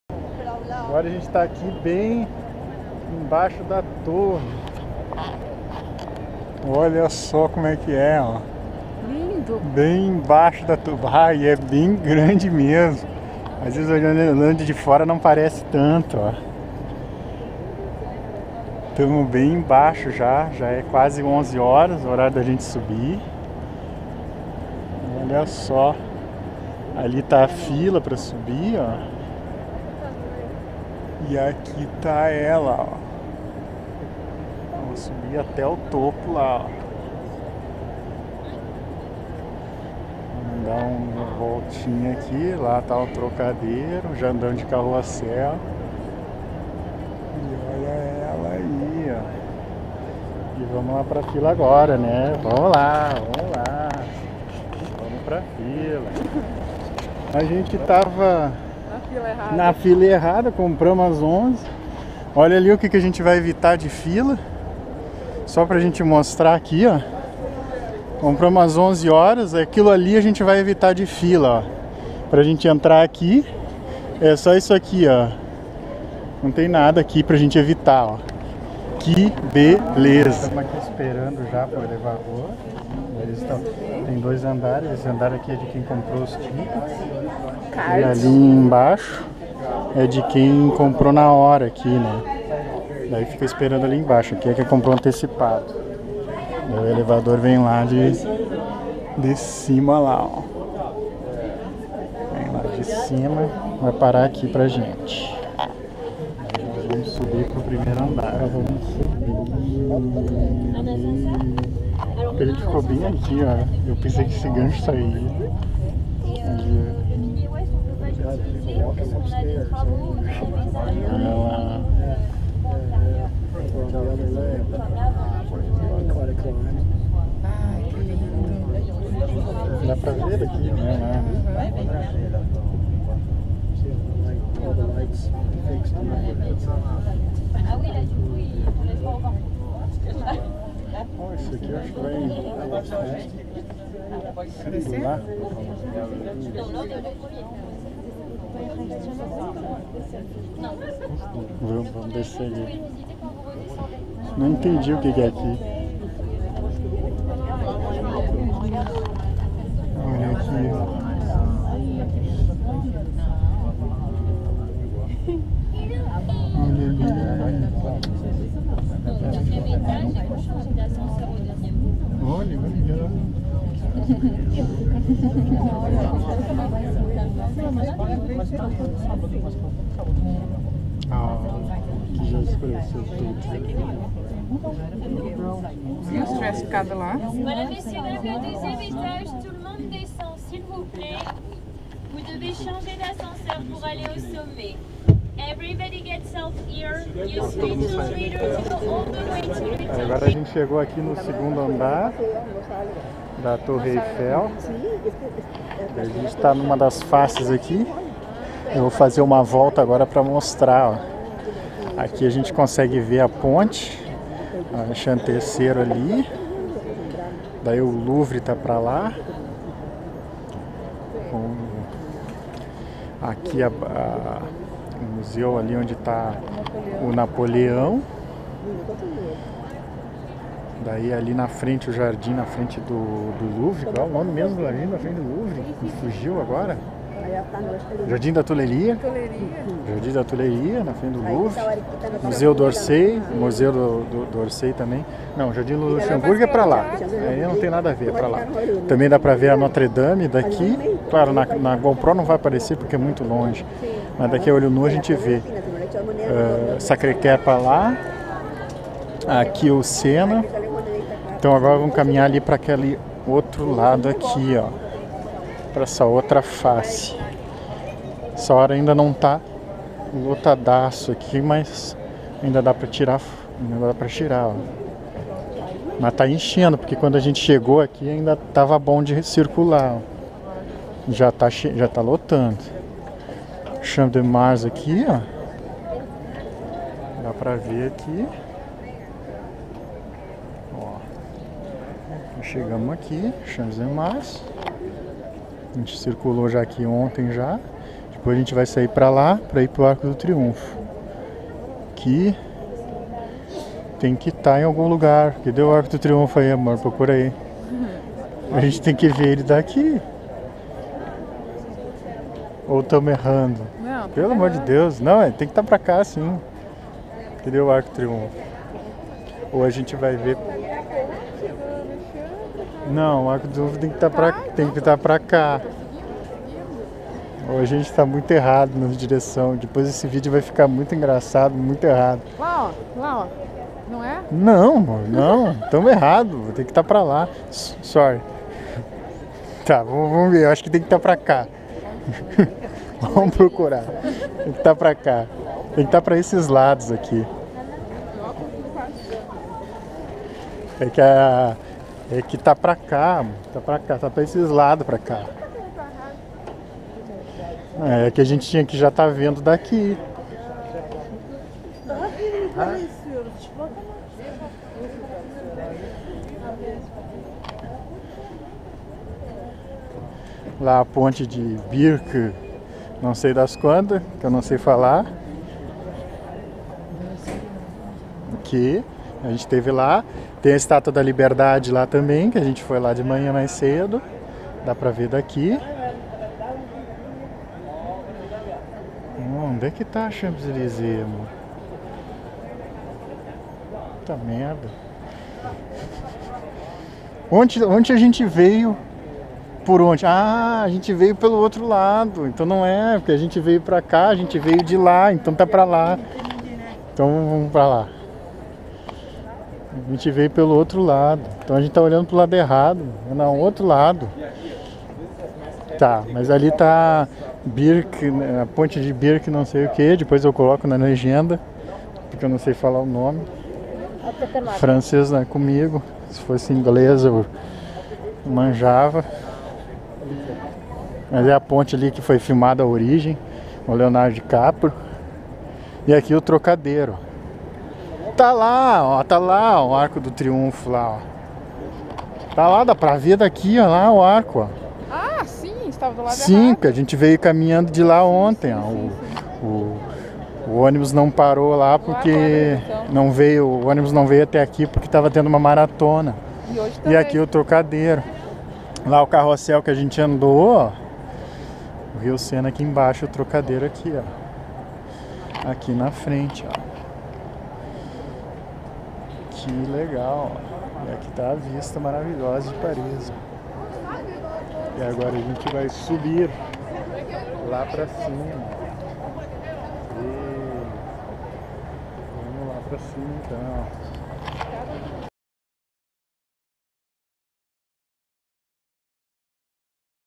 Paris 2017 - Torre Eiffel Subindo até 2º Andar
Subindo até o 2º andar da Torre Eiffel em 09/10/2017, segunda-feira.